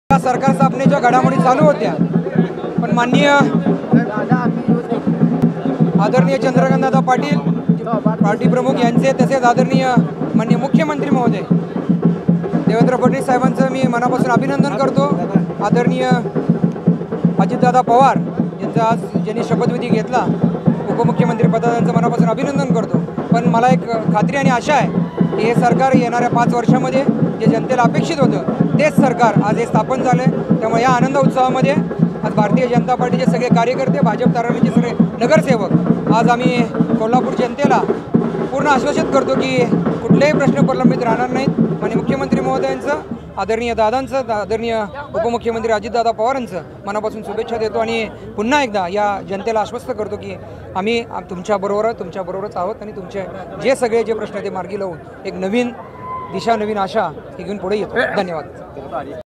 जनतेला अपेक्षित होतं तेच सरकार आलं -माजी आमदार अमल महाडिक
गेल्या महिनाभरापासून अनेक घडामोडी घडत गेल्या. मात्र जनतेला अपेक्षित होतं तेच भाजप सरकार सत्तेत आली आहेत. असं मत भाजपचे माजी आमदार अमल महाडिक यांनी व्यक्त केलं